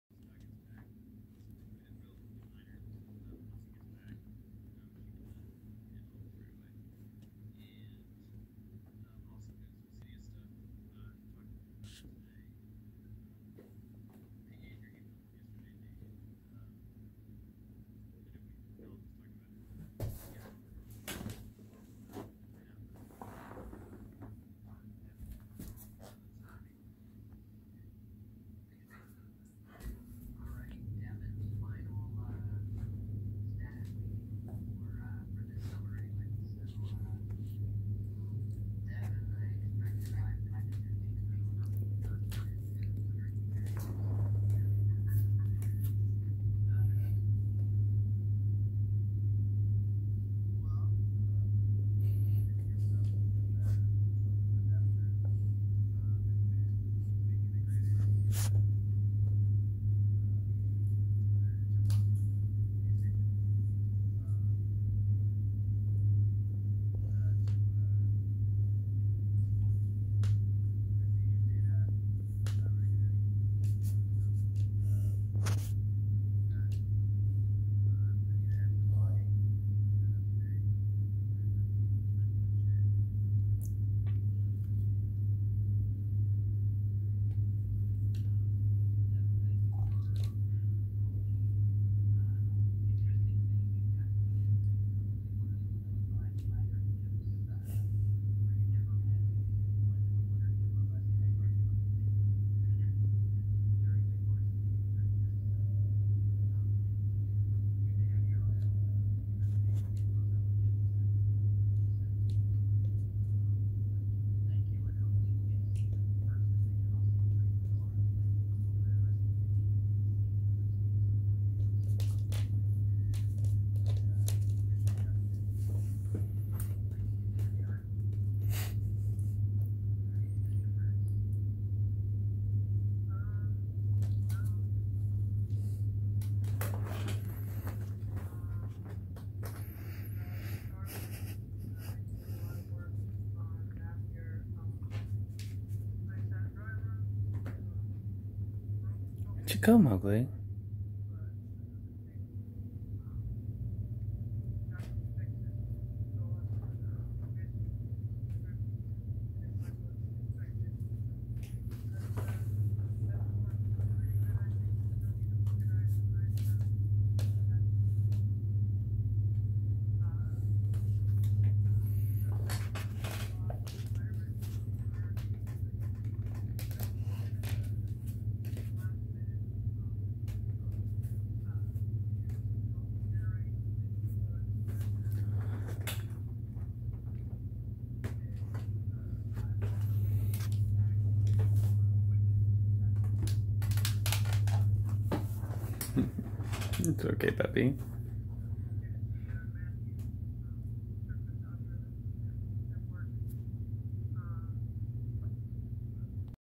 Mogli is baffled as Nala slowly disappears into his bed (21mos)
Mogli tries to play with his sister, but as she slowly gets deeper and deeper under the blanket he becomes perplexed at her witchcraft